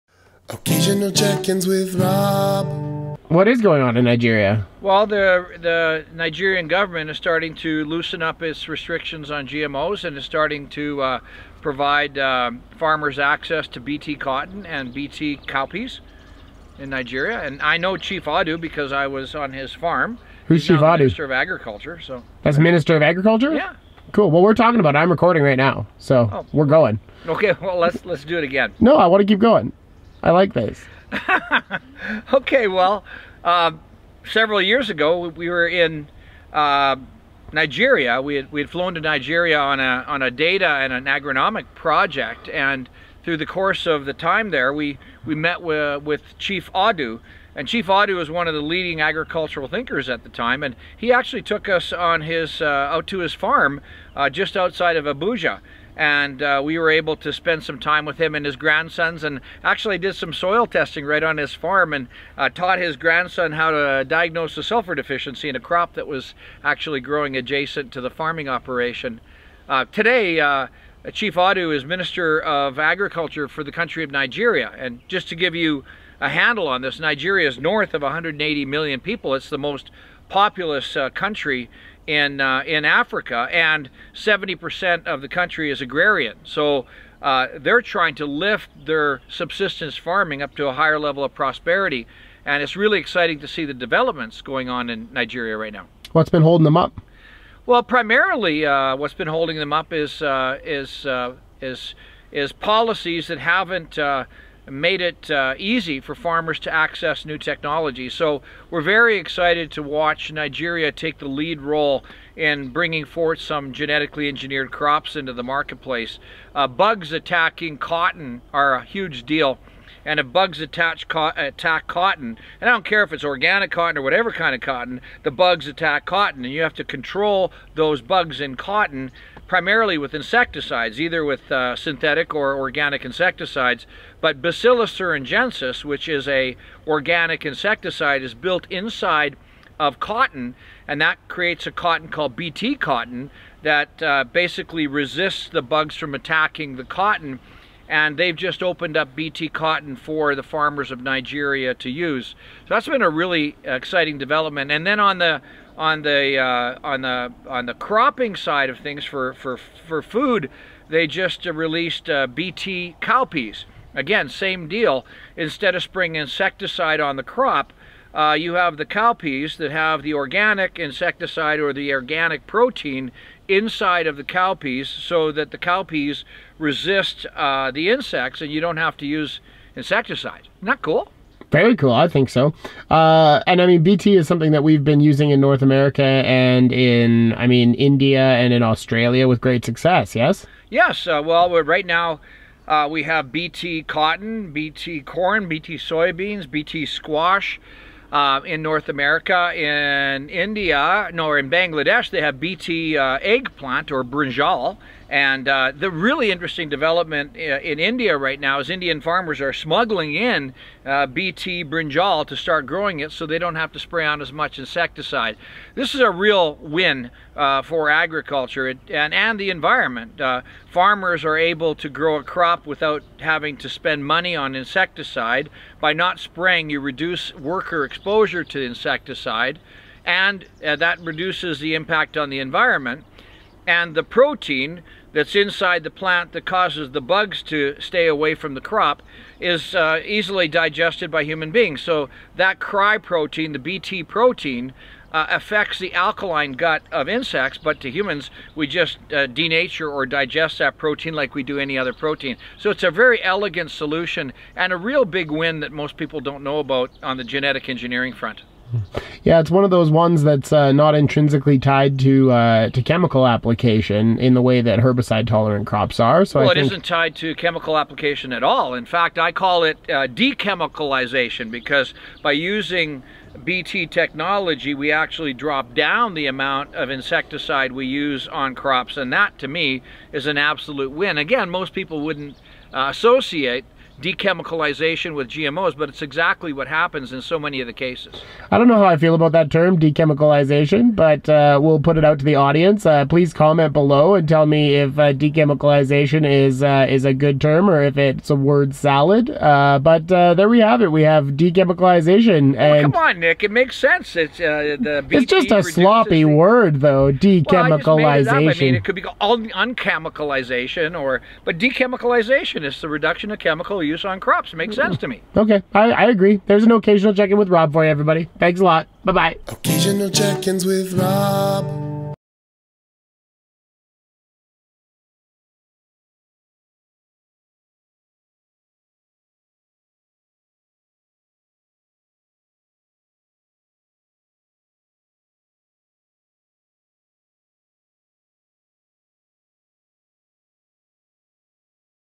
Nigeria and Biotech - Occasional Check Ins With Rob
In this installment of Occasional Check Ins With Rob, Nick and Rob discuss the biotech developments happening in Nigeria. Rob recounts the time he spent in Nigeria with the minister of agriculture.  Nigeria is starting to loosen up it's restriction on GMO crops, so BT cotton and cowpeas are soon to be grown in Nigeria. These crops have the ability to drastically curb chemical use.  Innovation in agriculture should be available to everyone.

This video was produced independently by Know Ideas Media.